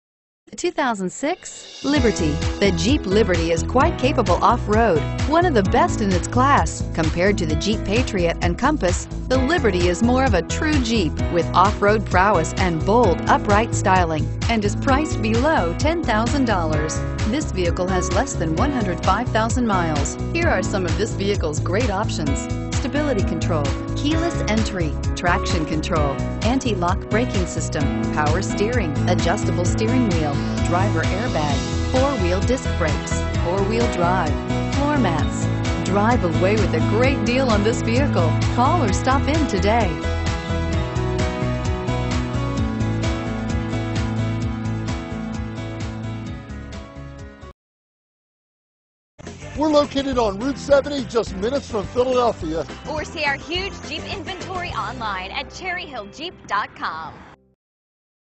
2006 Jeep Liberty Cherry Hill NJ 77164B
2006 Jeep Liberty Sport

You'll love this 2006 Jeep Liberty. This is a sport utility you'll want to take home. With 103566 miles, it features Automatic transmission and an exterior color of Black. Call and get in touch with Cherry Hill Chrysler Dodge Jeep Ram directly, and be the first to open the sport utility door today!